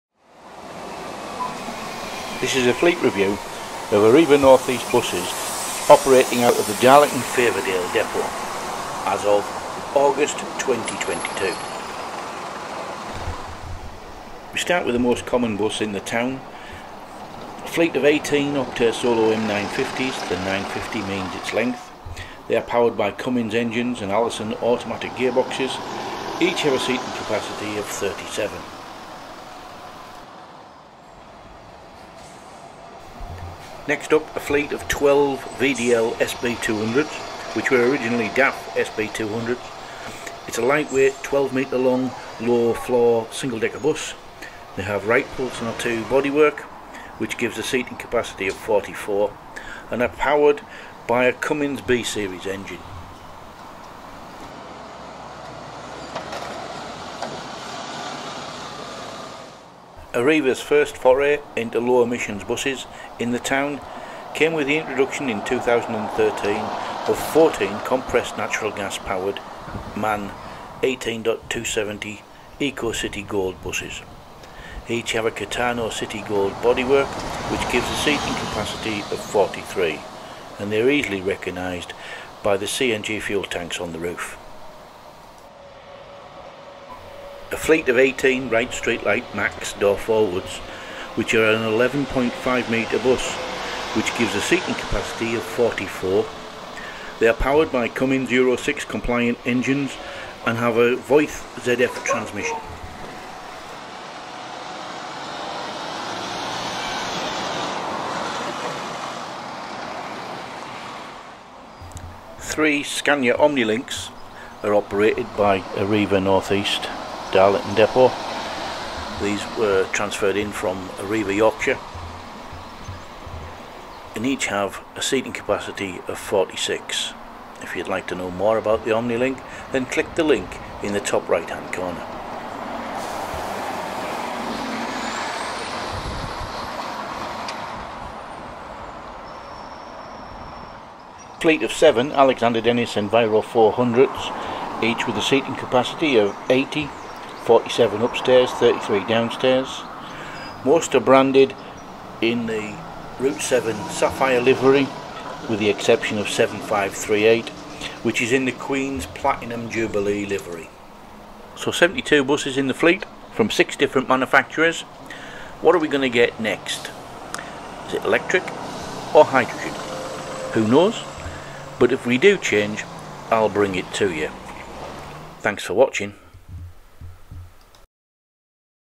Darlington Bus Fleet Review
Arriva Bus North East: Darlington depot fleet review.
We take a look at the bus fleet that is employed by Arriva Bus North East Darlington Depot.
Darlington Depot have a variety of buses from 6 different bus manufacturers and they operate over 70 buses.